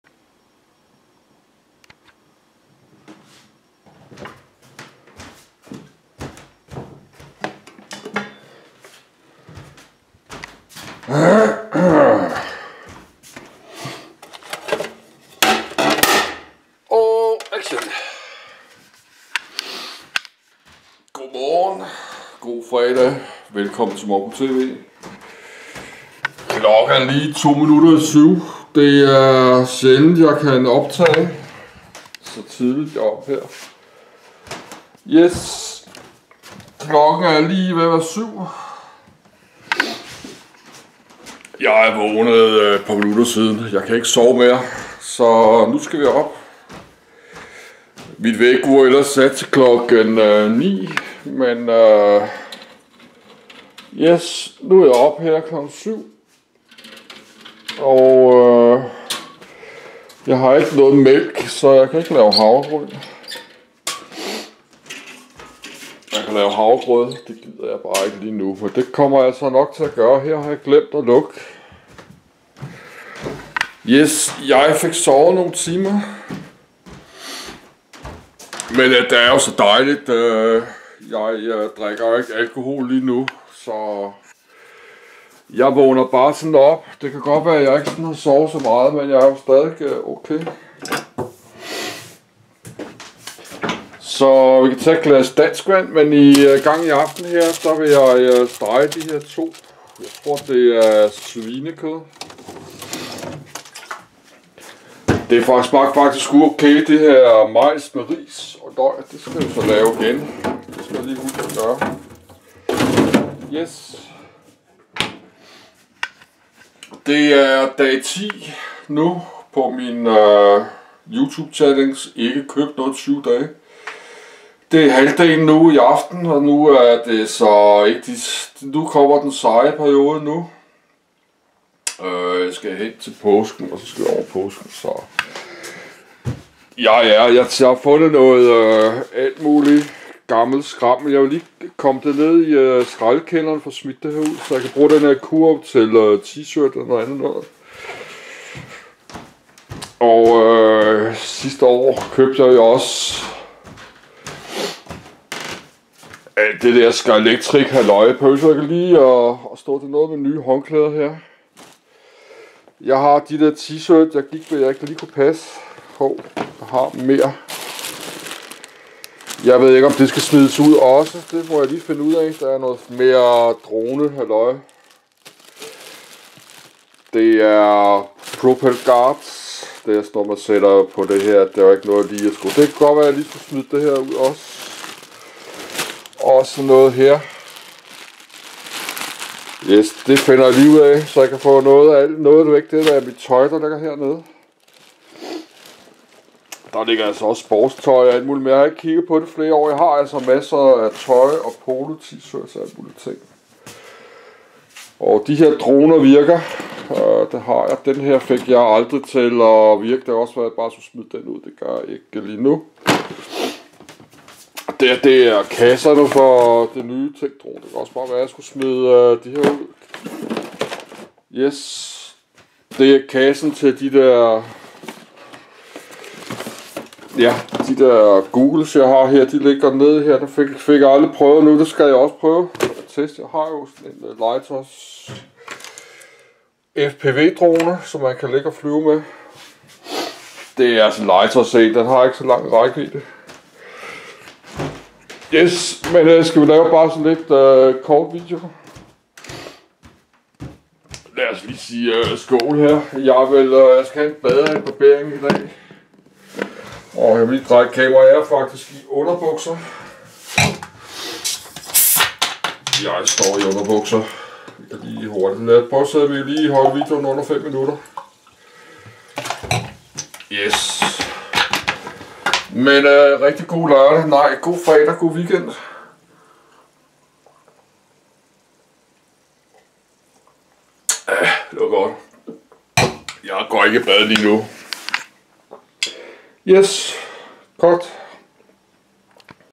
MongoTV_2404 - Godmorgen og God FREDAG 12.April 2019
Godmorgen og god FREDAG den 12.April. Jeg vågnet lidt i klokken 7 og kunne lige nå at optage lidt video FØR klokken 7 morgen, det er meget sjældent :-) Kan i have en god fredag også og weekend.